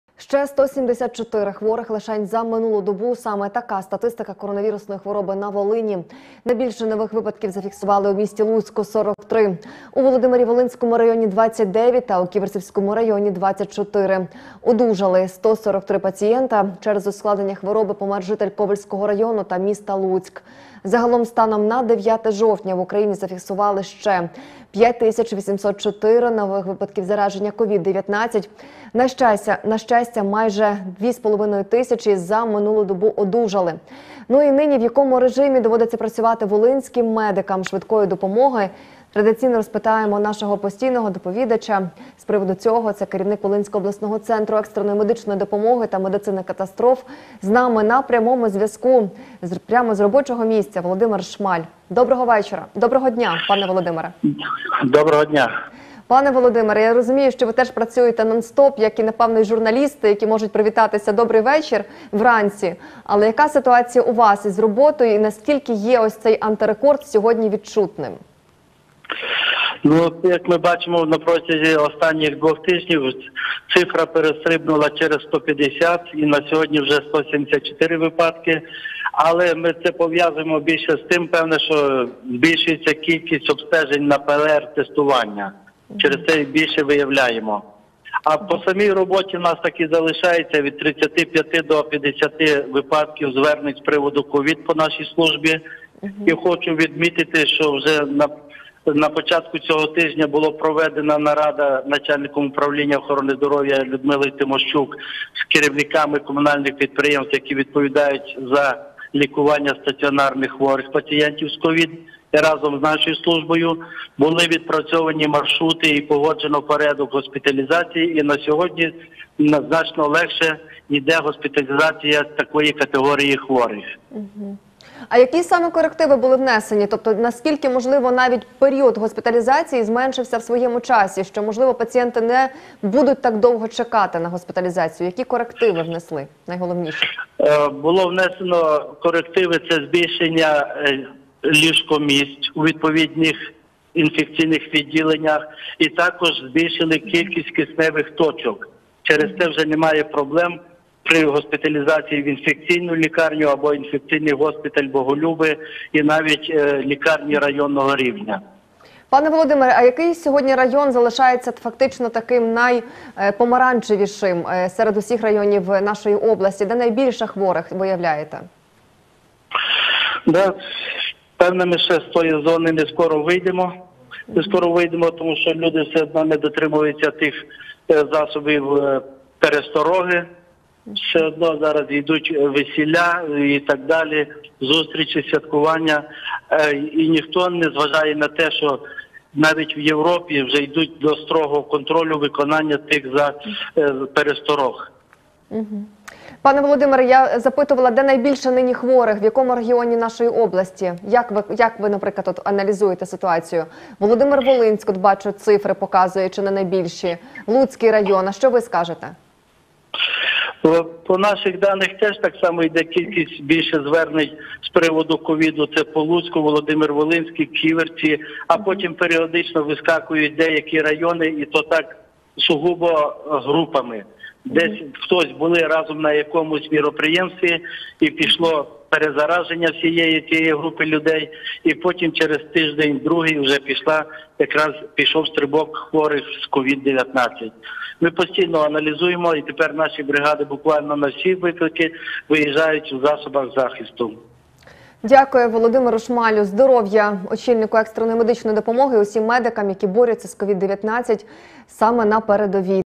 Двоє волинян померло та 174 захворіло: яка ситуація з коронавірусом на Волині
Ще 174 хворих лише за минулу добу. Така статистика коронавірусної хвороби на Волині. Найбільше нових випадків зафіксували у місті Луцьк - 43, у Володимир – Волинському районі – 29 та у Ківерцівському районі – 24. Одужали 143 пацієнта. Через ускладнення хвороби помер житель Ковельського району та міста Луцьк. Загалом, станом на 9 жовтня, по Україні зафіксували ще 5804 нових випадків зараження Ковід-19. На щастя, майже 2 з половиною тисячі за цю добу одужали.